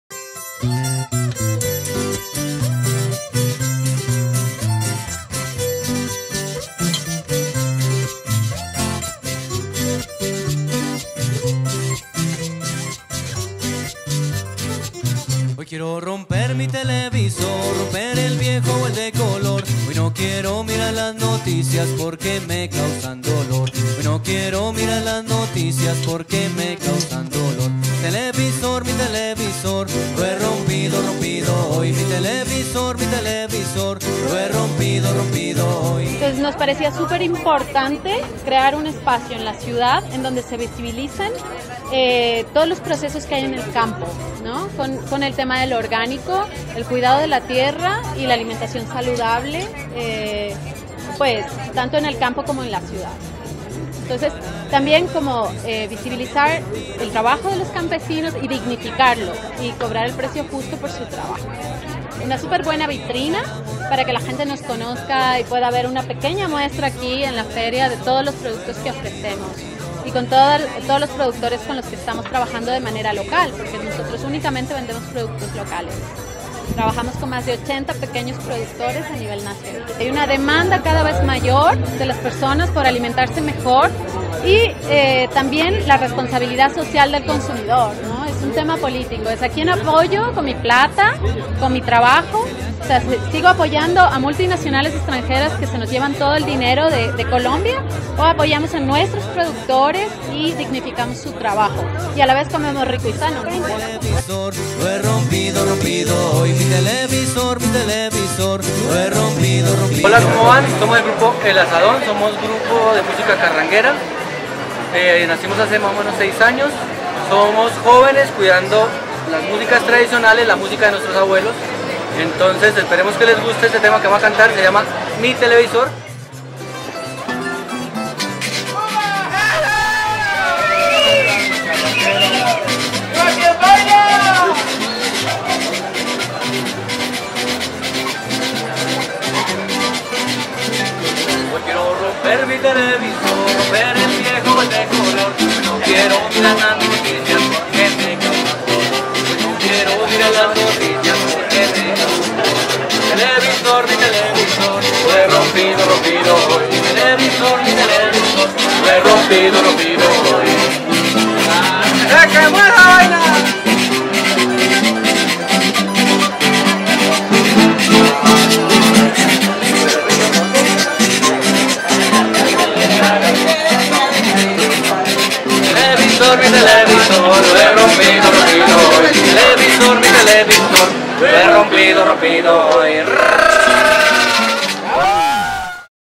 Festival gastronómico y cultural ‘Macarenazo’
Cientos de bogotanos, personas de otras regiones del país y extranjeros cumplieron la cita con el festival gastronómico y cultural más representativo del barrio La Macarena en Bogotá.